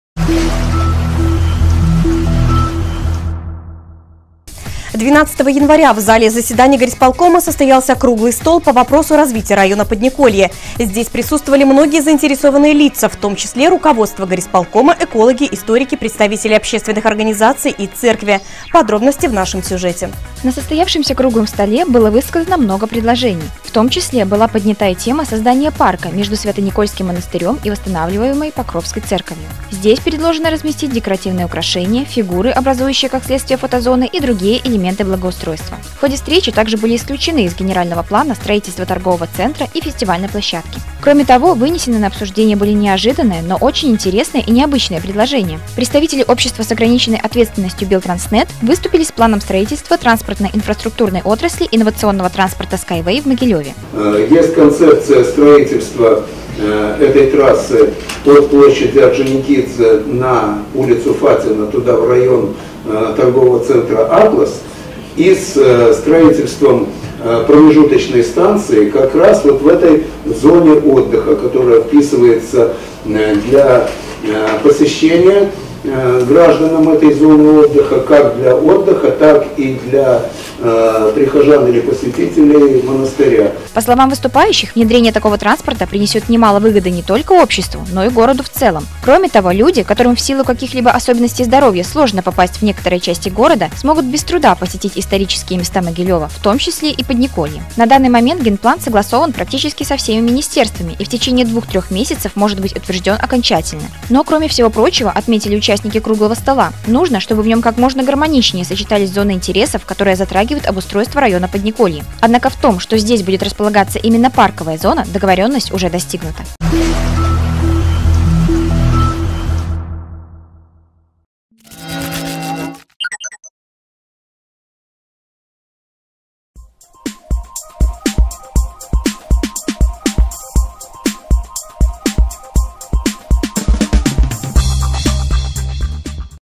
РАЗВИТИЕ SKYWAY В МОГИЛЁВЕ
12 января 2017 года в Городском исполнительном комитете Могилёва состоялось заседание по вопросу развития Подниколья — культурно-исторического района города, архитектурные достопримечательности которого внесены ЮНЕСКО в реестр наиболее ценных сооружений Европы. В заседании приняли участие чиновники, историки, экологи, представители общественных организаций и церкви, а также партнёры группы компаний SkyWay — ООО «Белтранснет», — которые представили общественности концепцию строительства городской транспортной линии от площади Орджоникидзе до улицы Фатина, с промежуточной станцией в Подниколье. По словам представителей компании «Белтранснет», предложенный проект идеально вписывается в городской ландшафт и способен в перспективе не только улучшить сообщение между городскими районами, но и способствовать росту туристической отрасли региона. В настоящий момент компания «Белтранснет» проводит анализ общественного мнения, целью которого является создания оптимального маршрута трассы с учётом всех пожеланий могилевчан. Напомним, что представители Могилёва уже знакомы с технологией SkyWay: первый заместитель председателя Могилёвского городского исполнительного комитета Александр Потёмкин и председатель Могилевского городского совета ветеранов Фёдор Матьков были в составе делегации, посетившей ЭкоТехноПарк в декабре 2016 года. Кроме того, региональная газета «Вестник Могилёва» разместила на своих страницах публикацию под названием «Дорога будущего, или билет на юнибус», посвященную Струнному транспорту Юницкого.